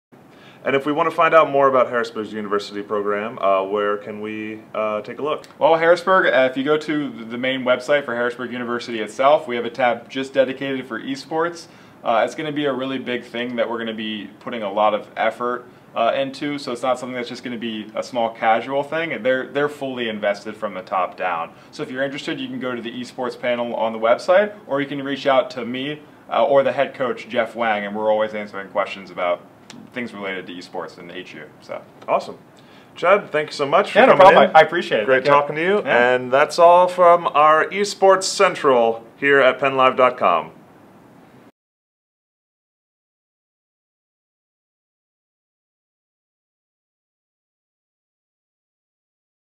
More info about Harrisburg University esports
Chad Smeltz, director of the Harrisburg University esports program, answers our questions about Fortnite, Overwatch, League of Legends and Hearthstone. (Video by Sean Adams/PennLive)